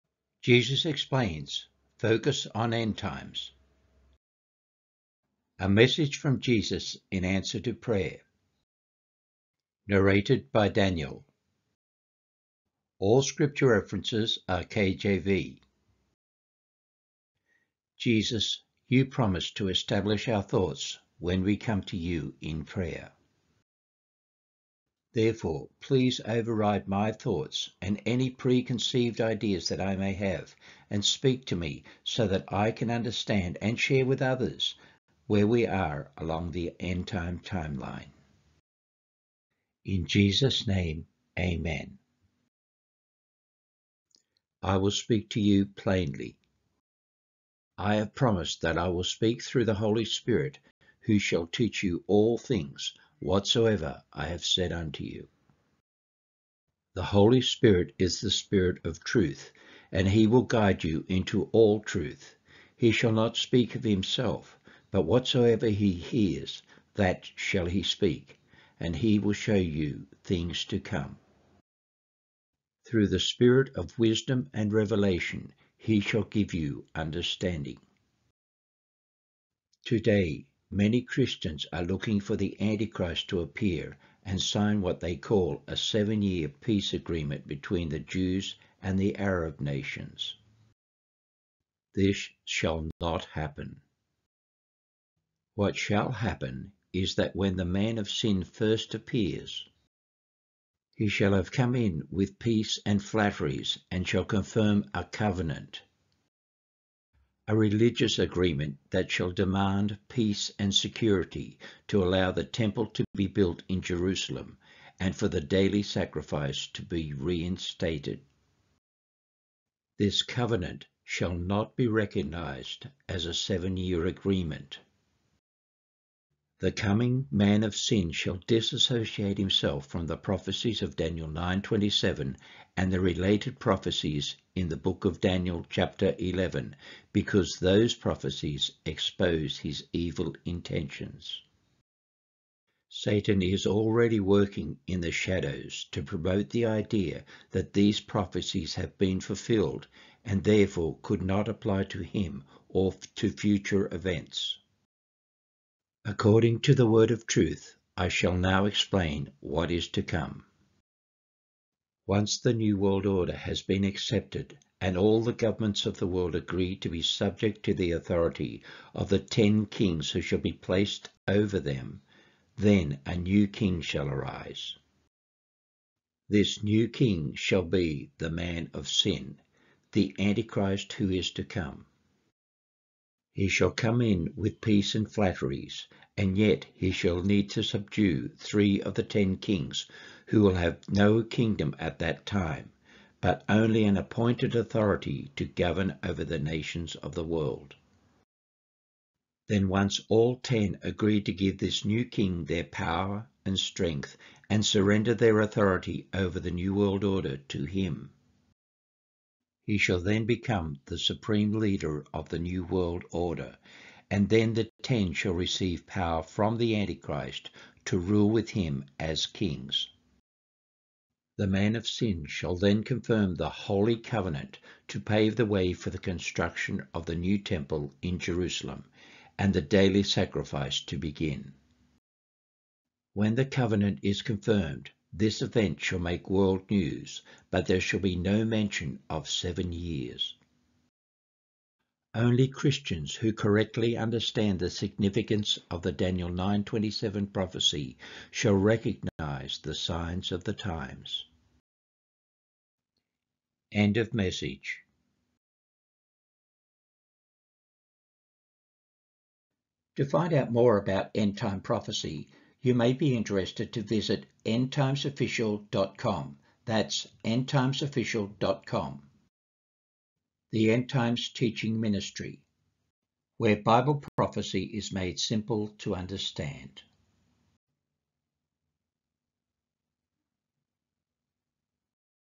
REVELATION & PROPHECY | FOCUS ON END TIMES!
IN THIS VIDEO Jesus explains what we can expect to see happen in the not-too-distant future – according to prophecy.

IN ANSWER TO PRAY Jesus explains how we can expect to see the rise of the New World Order with a council of 10 “kings” who shall gain power and authority to rule over ALL the governments of the world in the form of a One World Government. (Dan8:23 Rev13:1)

IN A MESSAGE FROM JESUS delivered through the Holy Spirit, Jesus explains how the “man of sin” (the Antichrist) shall be revealed and arise to power through “peace and flatteries” to become the supreme leader of the New World Order – and how he will then confirm a “holy” covenant that shall usher in an era of “Peace and Security” that will allow the Jews to rebuild their temple in Jerusalem and resume their daily sacrifice. (Jn16:13 2Thess2:3 Dan11:21 Dan7:23-24 Dan9:27 Rev17:12-13 Rev11:1-2 Dan8:13-14 KJV)